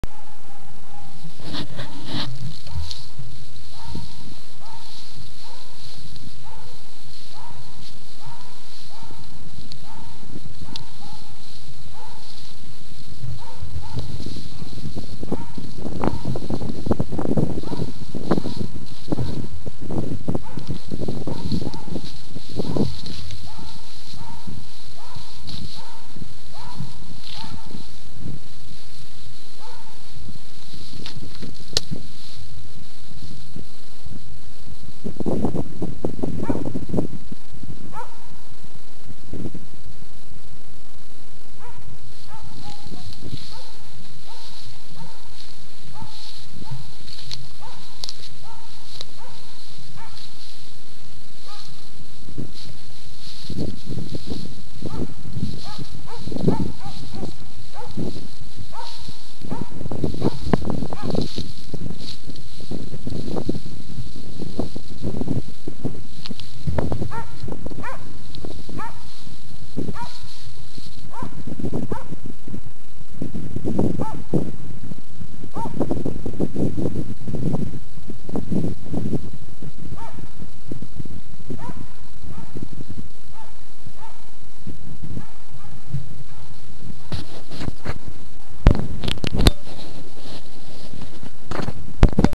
gammelhunden
10 åriga Ida kämpar på i år också,detta var bara träning,annars hadde det smällt